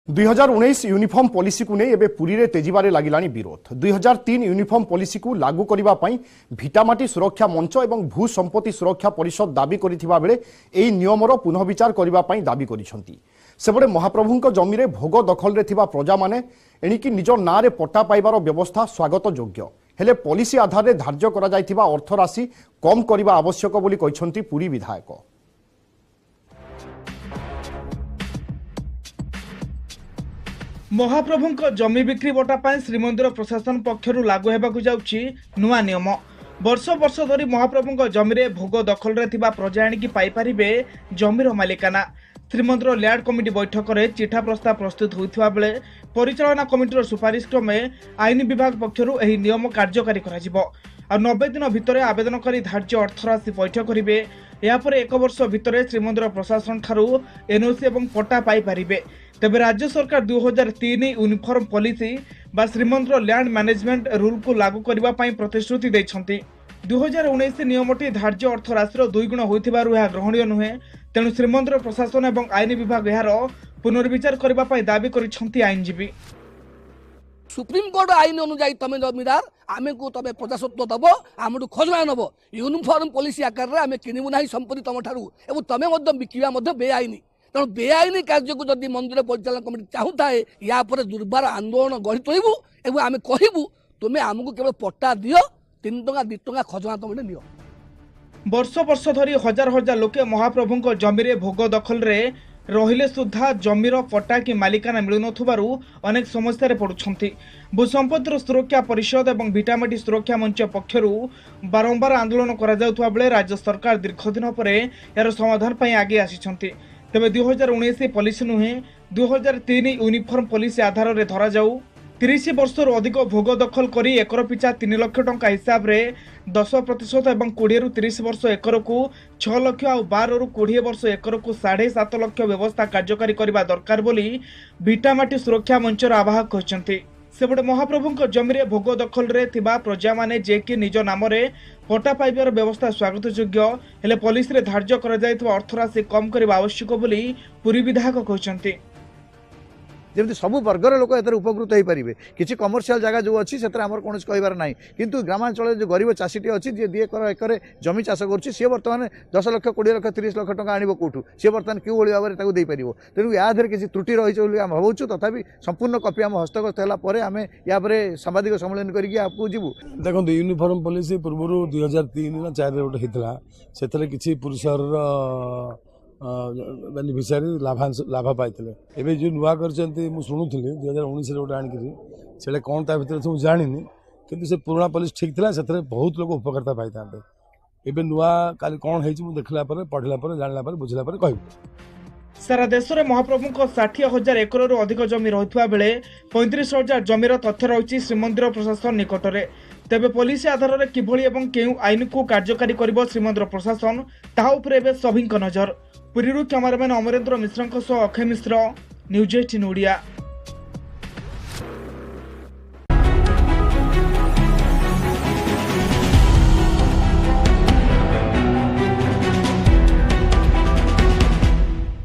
Jagannath Dham | ୨୦୦୩ ୟୁନିଫର୍ମ ପଲିସିକୁ ଲାଗୁକରିବାକୁ ଦାବି | Odia News
ଭିଟାମାଟି ସୁରକ୍ଷା ମଞ୍ଚ ୨୦୦୩ ୟୁନିଫର୍ମ ପଲିସିକୁ ଲାଗୁକରିବାକୁ ଦାବି କରିଛି